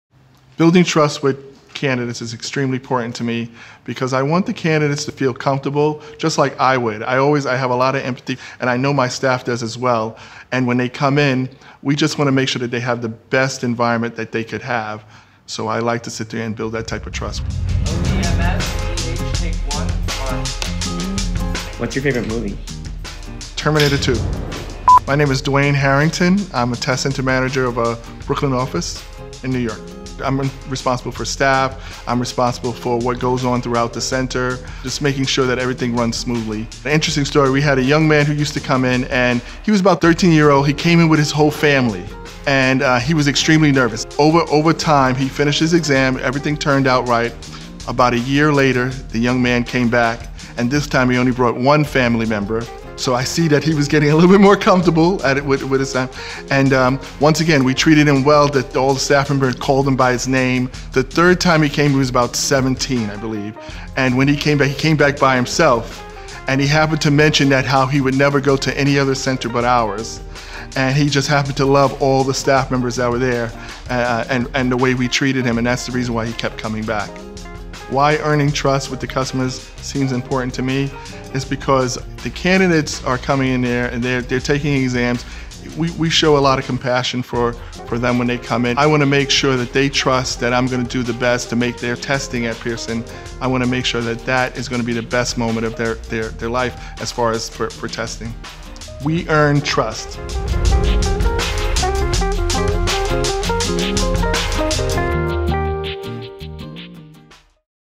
Pearson Purpose | Our Values, Dwayne's Story
Dwayne Harrington, Test Center Manager in our Assessments & Qualifications division, understands that acting with empathy and compassion leads to candidates having the best testing environment possible.